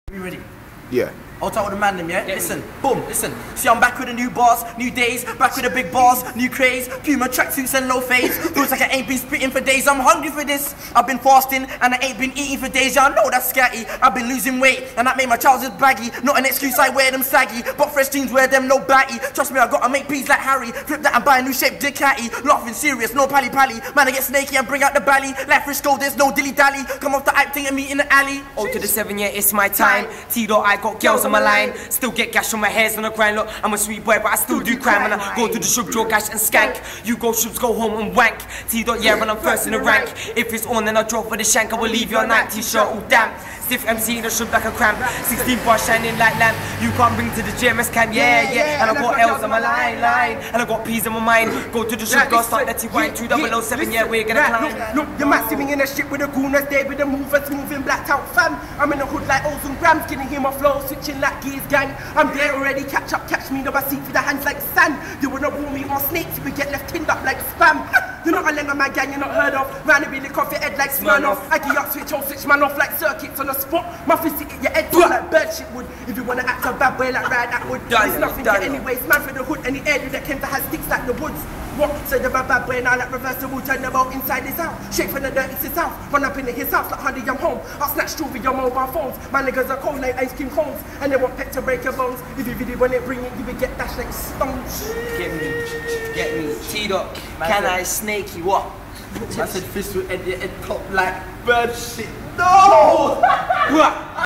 Snakeyman, Cani, T Dot Roadside Freestyle
Cani T Dot and Snakey spittin heat